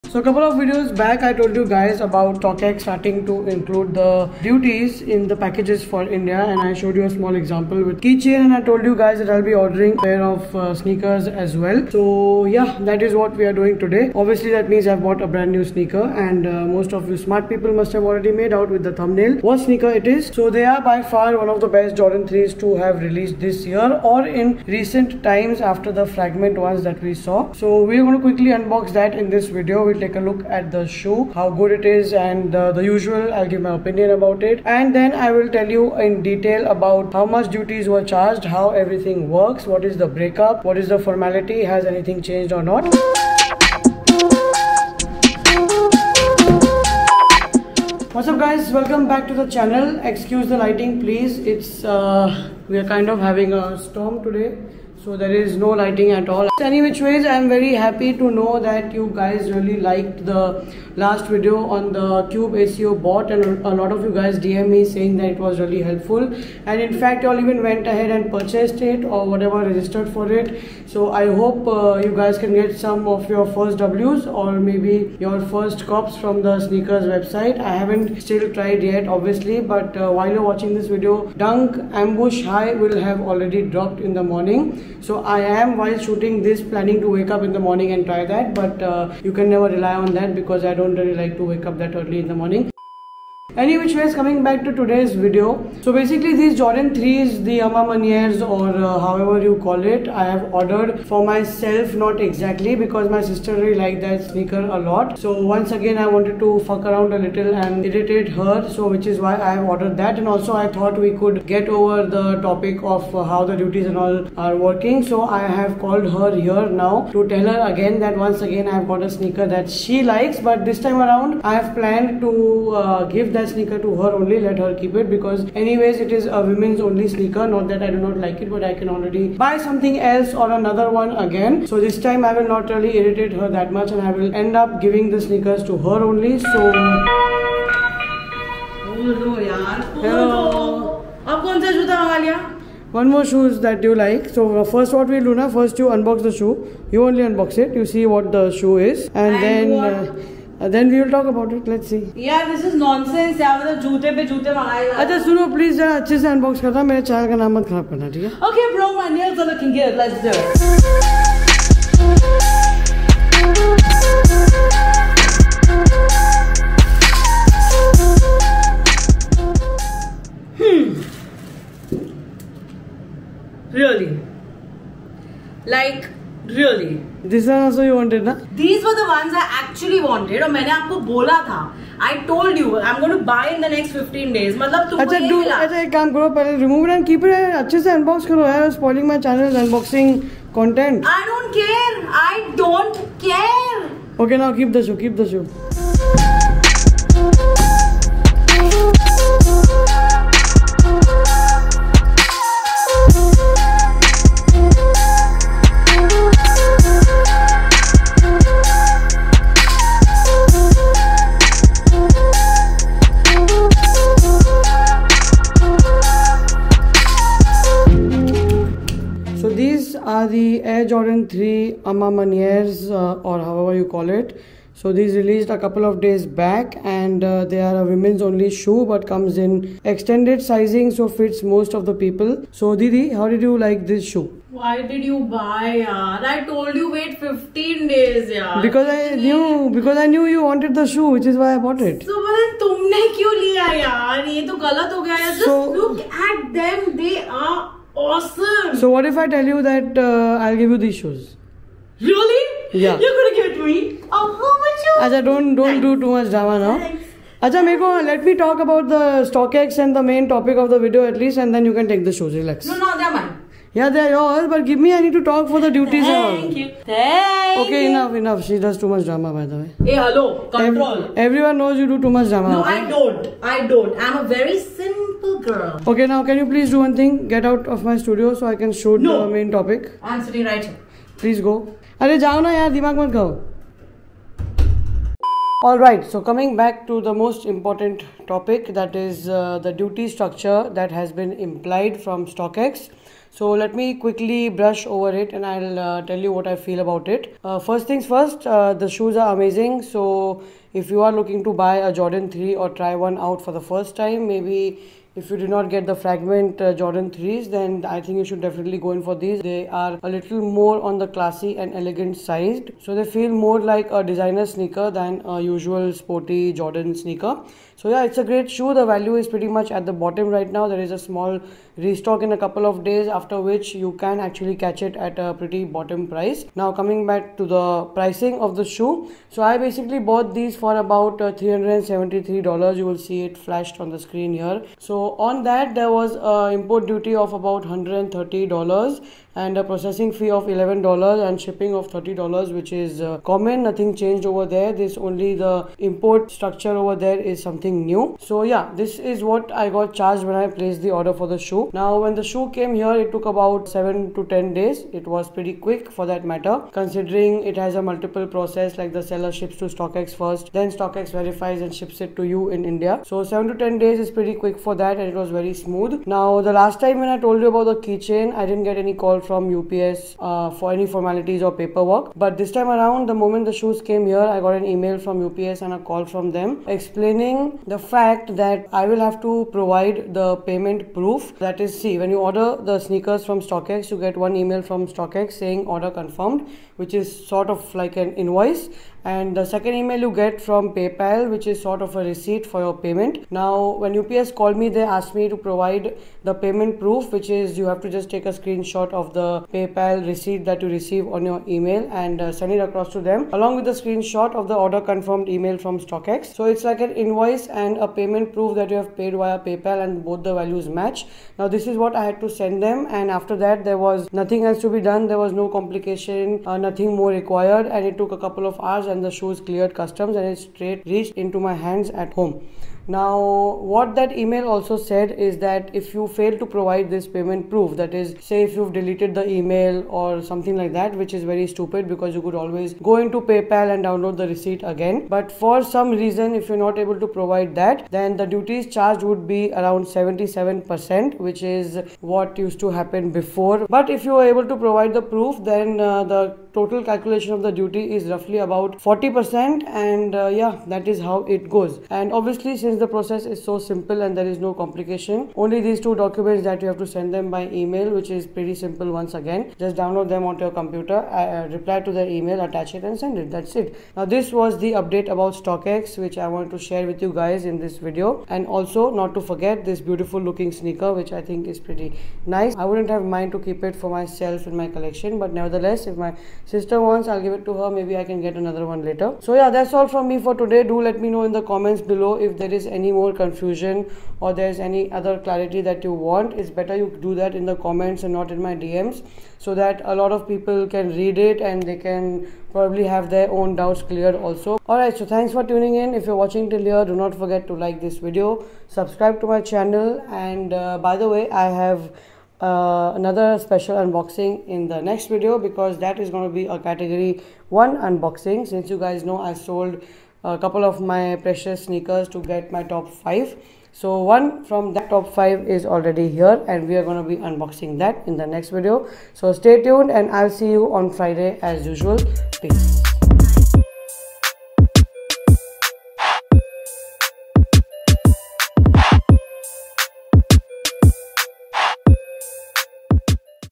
PRANKING MY SISTER WITH HER FAVOURITE JORDAN’s | Stockx India Custom Duties Update
Just a small prank on my sister with the Jordan 3 AMA Maniere. Hope you like it. Also update about stockx india custom duties update.